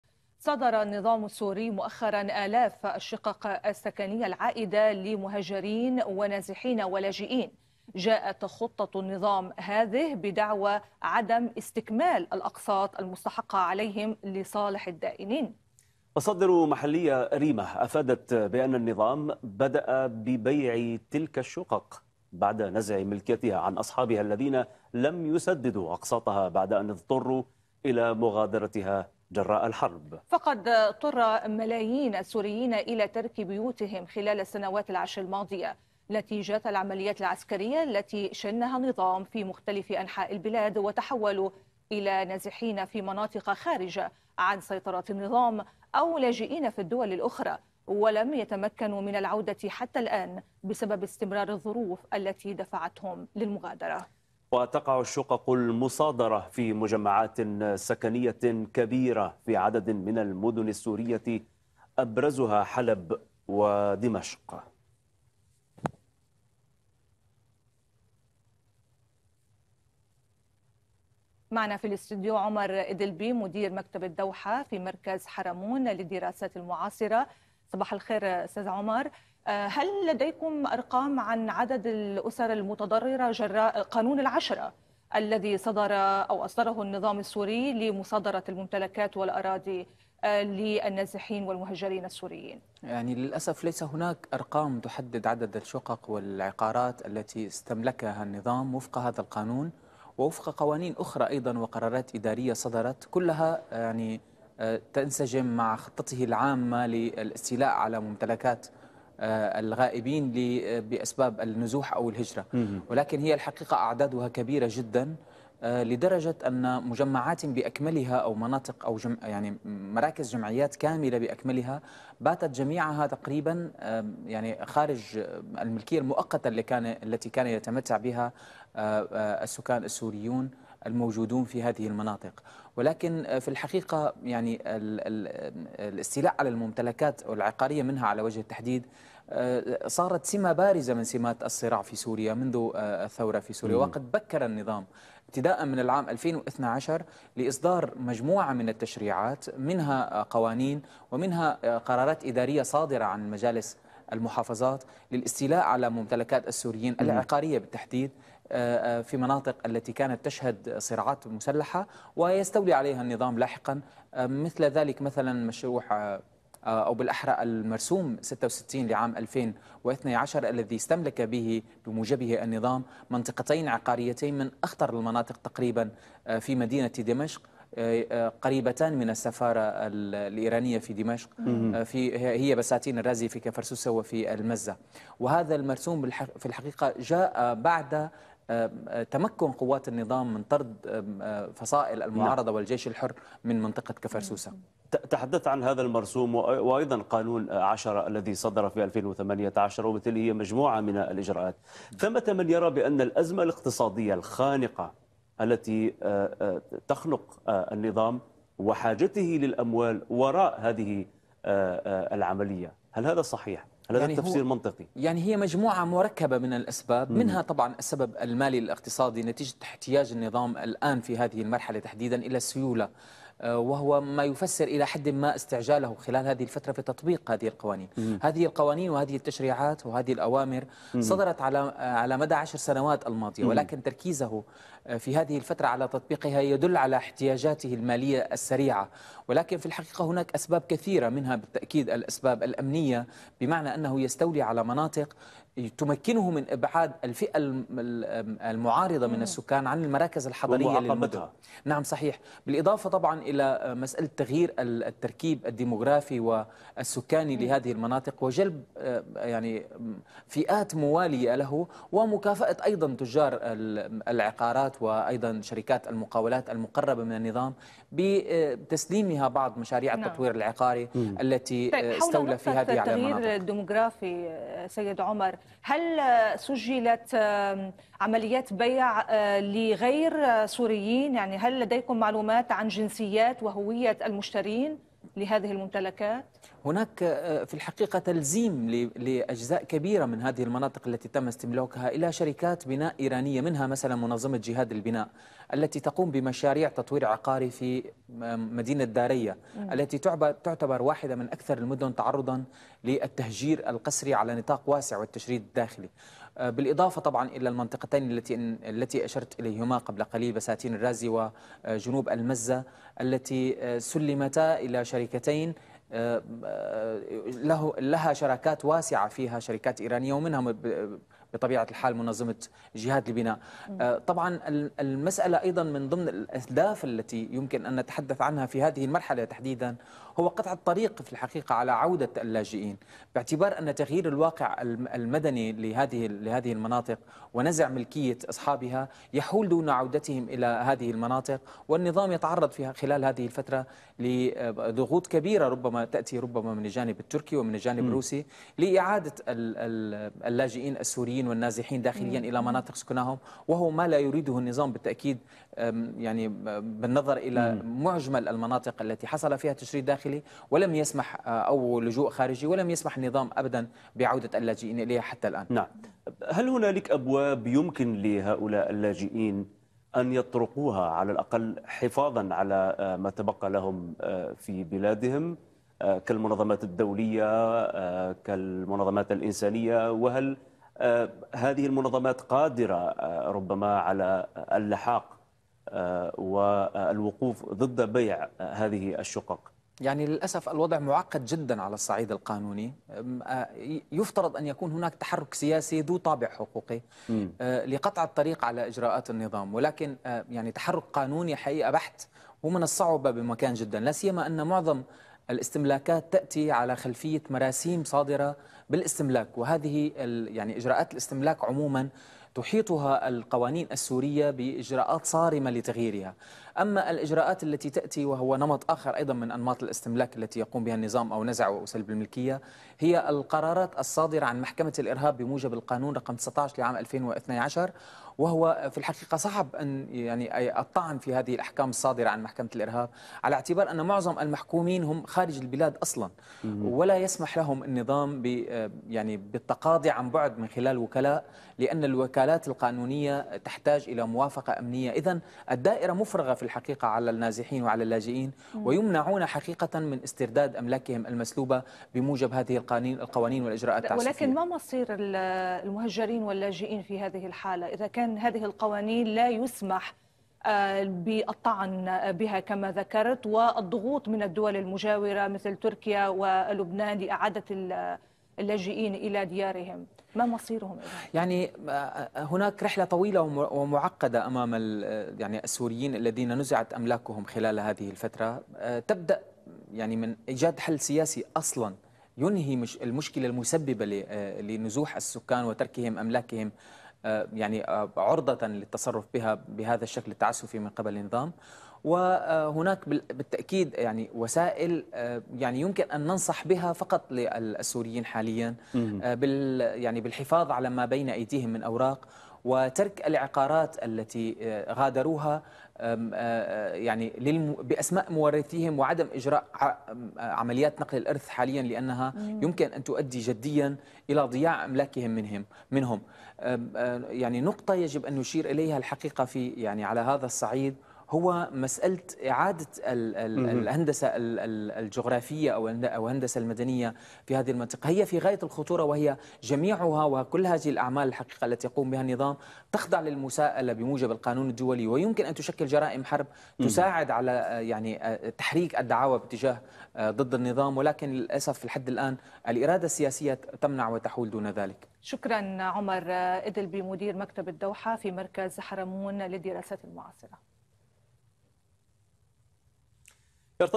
النظام السوري يسطو على بيوت اللاجئين والنازحين ويكافئ بها داعميه
صادر النظام السوري الآلاف من الشقق السكنية العائدة لمهاجرين ونازحين ولاجئين بدعوى عدم استكمال الأقساط المستحقة لدى الدائنين، وتقع في مجمعات سكنية كبيرة في عدد من المدن السورية ابرزها في حلب ودمشق.